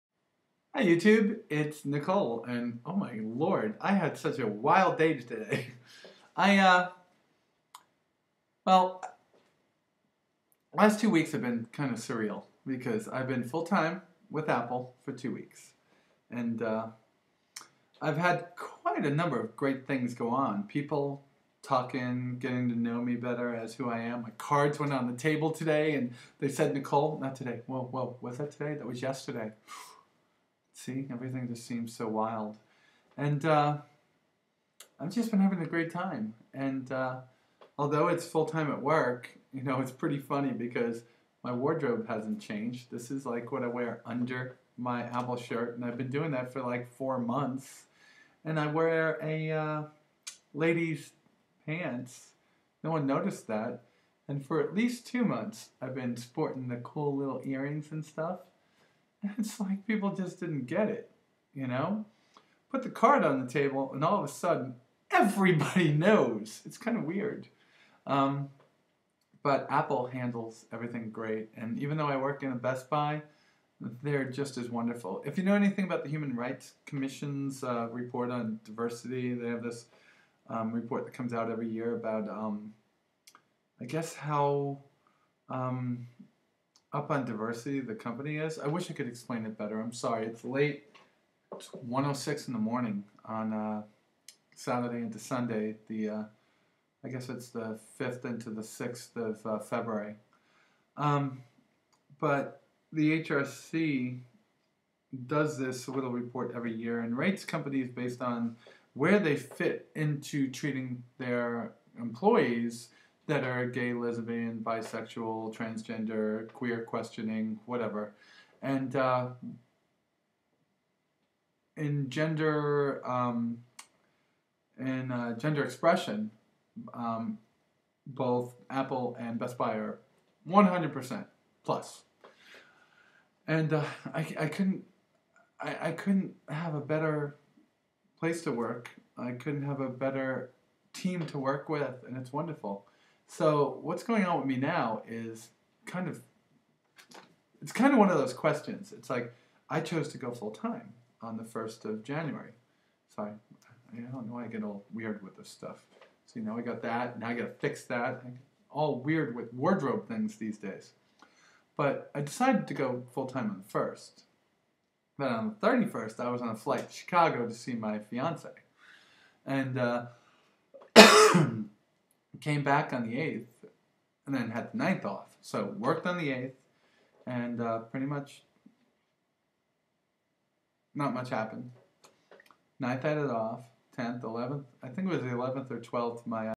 2 Weeks Full Time
Just finishing up my first two weeks as Nichole Full Time.  I just rant a bit in this video.  Sorry but there is some content hidden in there that is good for all.